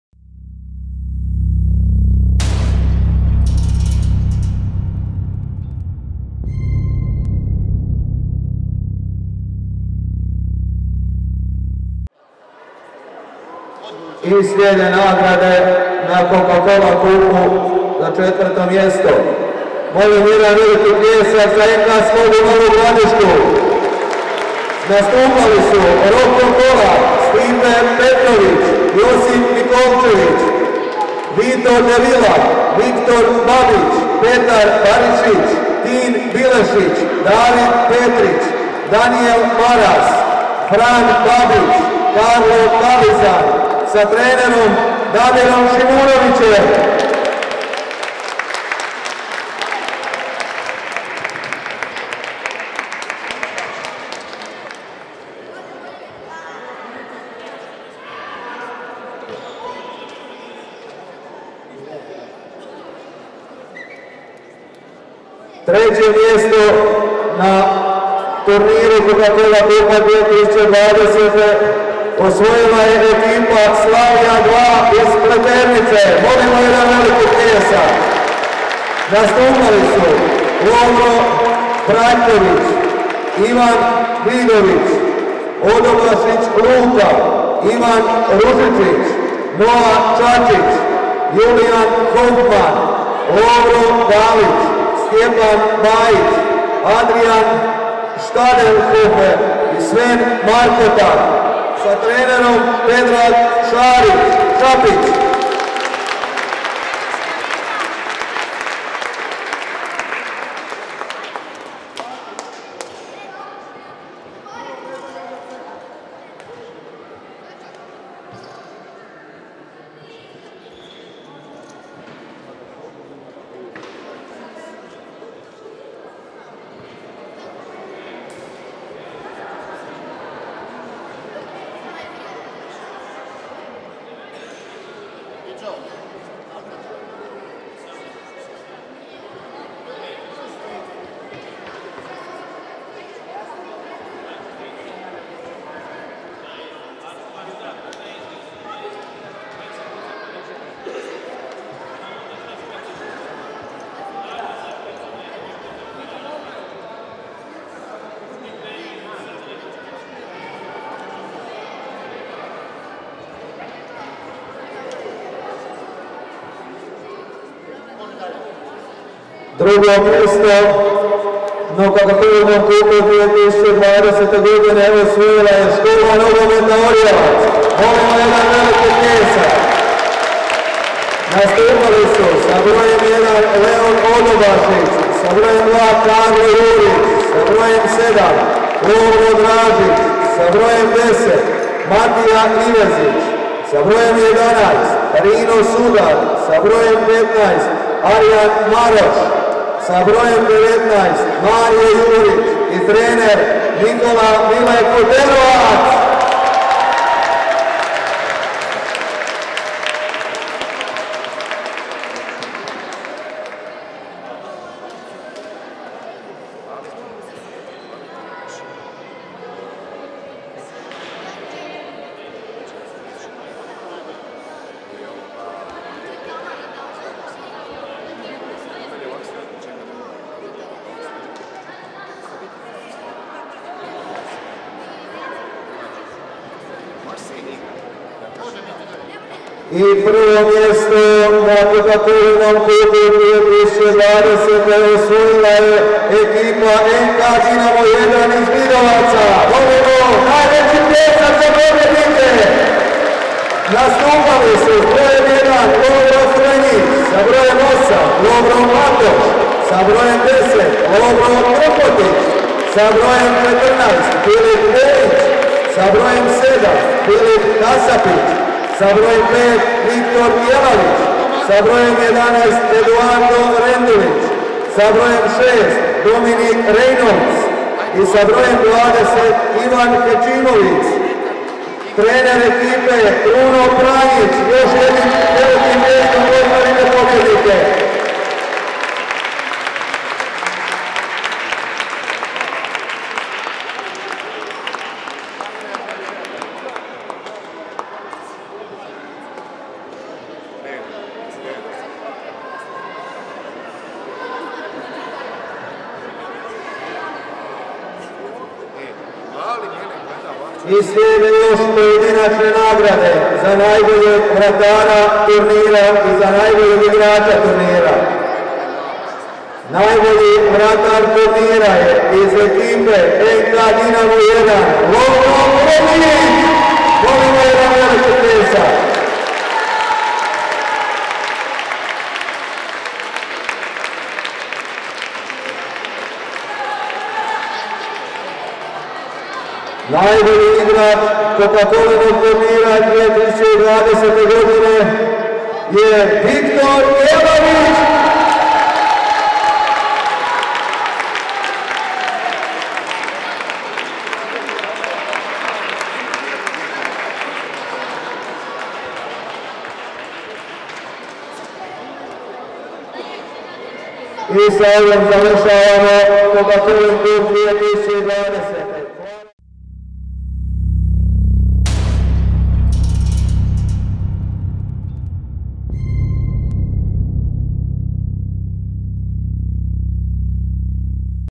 Batrina Turnir 2020 # Mlađi Pioniri # Dodjela medalja
Nogometni turnir u Batrini održan je u 8.2.2020. godine.

Dodjela medalja!

Iznad Svih!

Dinamo!